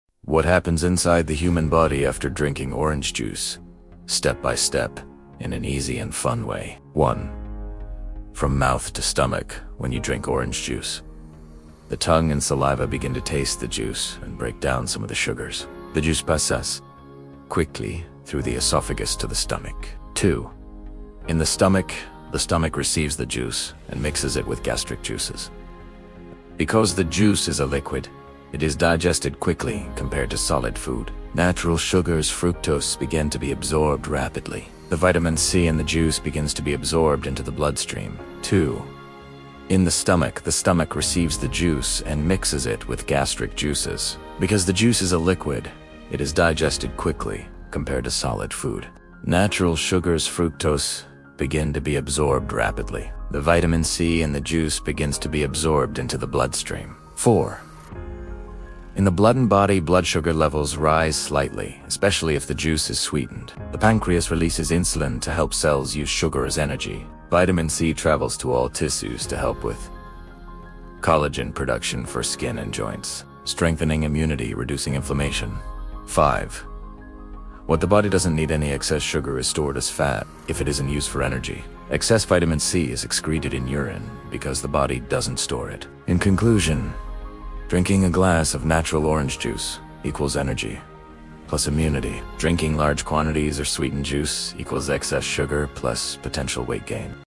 What Happens Inside Your Body After Drinking Orange Juice 🍊 | Amazing Science Explained!
Have you ever wondered what happens inside your body after drinking a glass of orange juice? 🍊
In this short scientific video, we explore how your body digests, absorbs, and benefits from the natural vitamins, sugars, and antioxidants in orange juice.
Discover the journey of vitamin C through your bloodstream, how it boosts your immune system, and why orange juice is more powerful than you think!

🎥 Realistic scientific animation
🎙️ Educational voice-over
📺 Perfect for science lovers and health enthusiasts